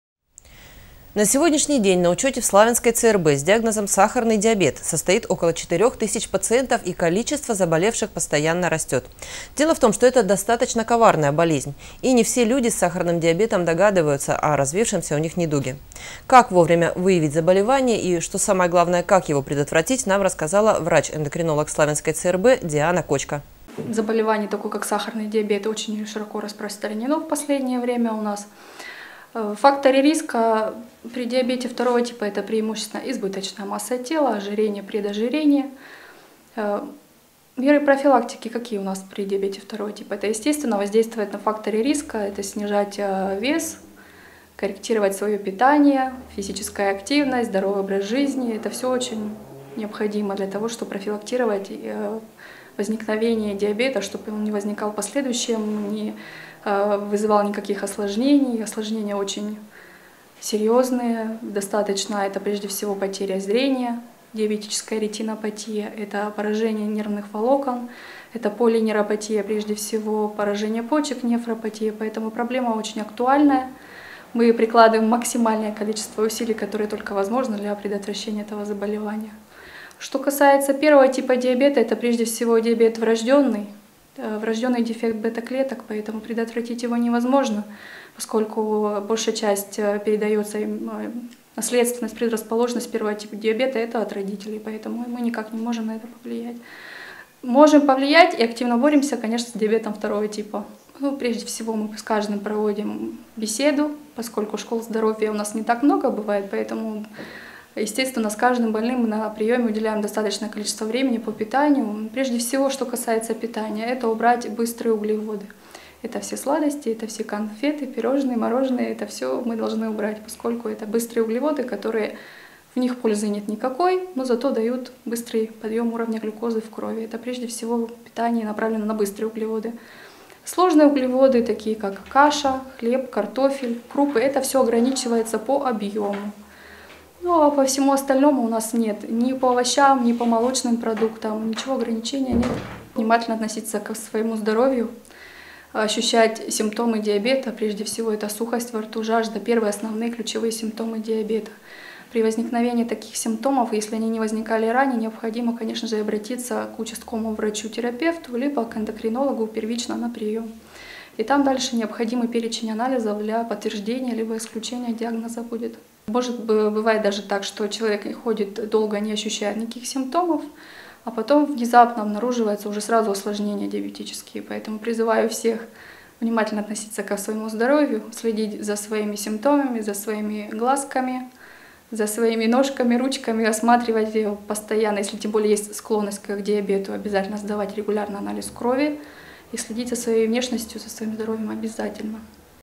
Сахарный диабет
На сегодняшний день на учете в Славянской ЦРБ с диагнозом- сахарный диабет состоит около 4 тысяч пациентов и количество заболевших постоянно растет. Дело в том, что это достаточно коварная болезнь, и не все люди с сахарным диабетом догадываются о развившемся у них недуге. Как вовремя выявить заболевание, и, что самое главное, как его предотвратить, нам рассказала врач-эндокринолог Славянской ЦРБ Диана Кочка.